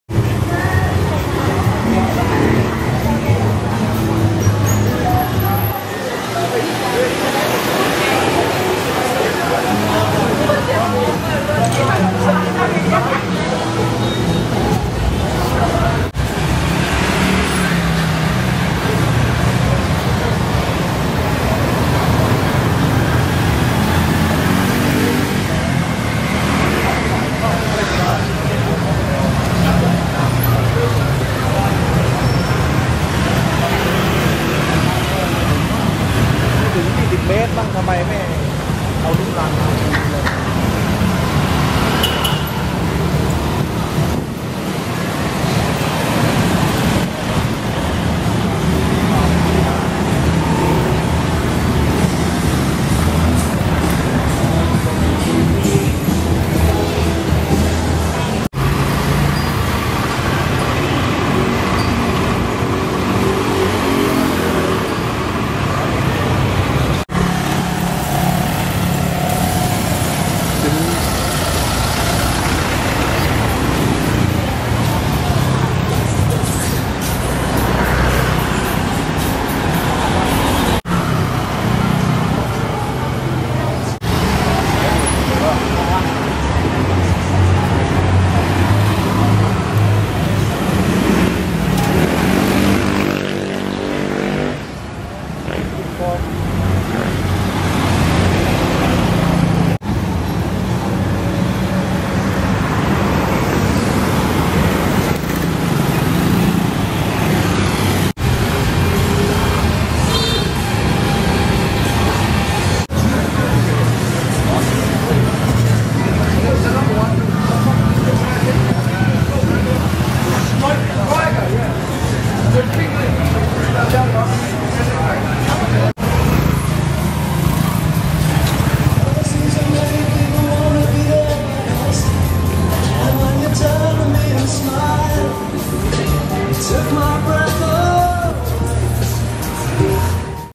Soi Buakhao, Pattaya - July 2015
A short video showing a few sights on Soi Buakhao in Pattaya, Thailand. I went to try to video the Swan Pook Ladyboy bar, but it was completely dead with nothing going on.

 For this video I wasn't sure of the area (and was also a bit drunk) so that accounts for the random shots of the street. If I ever do any more future Pattaya trips I will do a more comprehensive video as it's actually a very long street with lots of bars, guesthouses and restaurants around.

 View in HD for better quality.